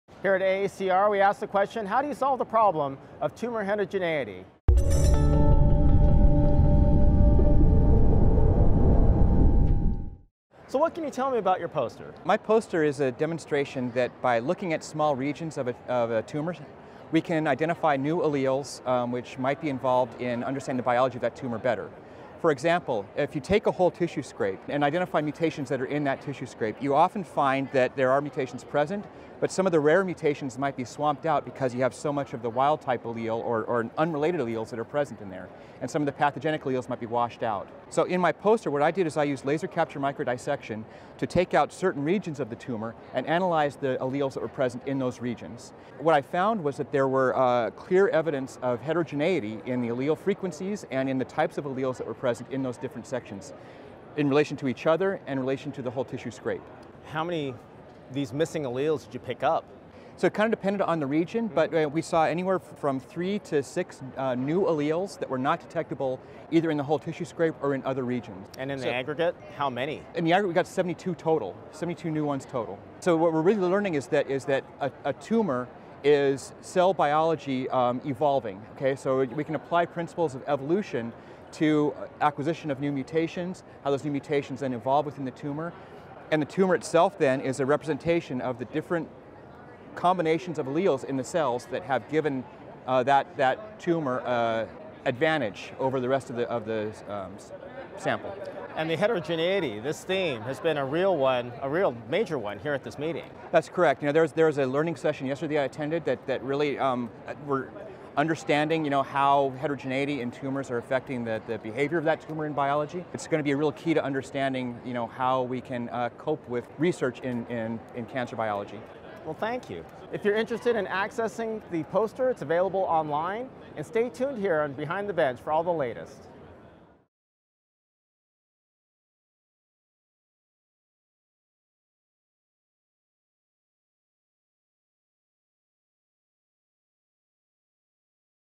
Identifying Novel Alleles using LCM and NGS | AACR 2015 Poster
A major theme at this year’s American Association for Cancer Research meeting has been tumor heterogeneity, and Stephen Jackson (Thermo Fisher Scientific) presents a poster entitled “Identification of rare and novel alleles in FFPE tumor samples using laser capture microdissection (LCM) and Ion AmpliSeq™ sequencing technologies”. In this video interview Stephen shares a few highlights of his poster. After AACR15 download the poster